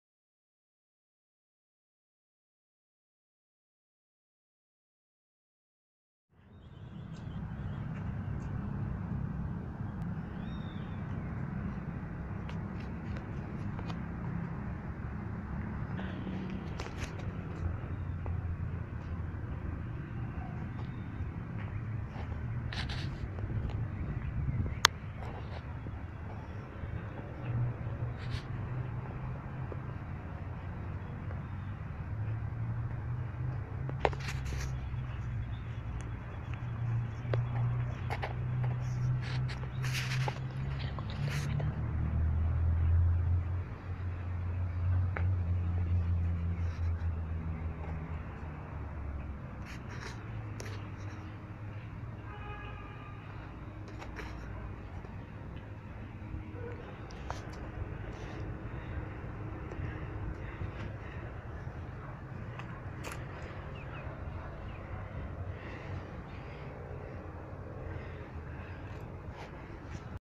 El Gran Capitán tren de pasajeros Máquina 7927 VICTORIA. Ferrocarril General Urquiza.
Breve vídeo del pasado y presente de Victoria, la Máquina 7927 de el tren de pasajeros El Gran Capitán.
Agradezco a Radamel por el vídeo!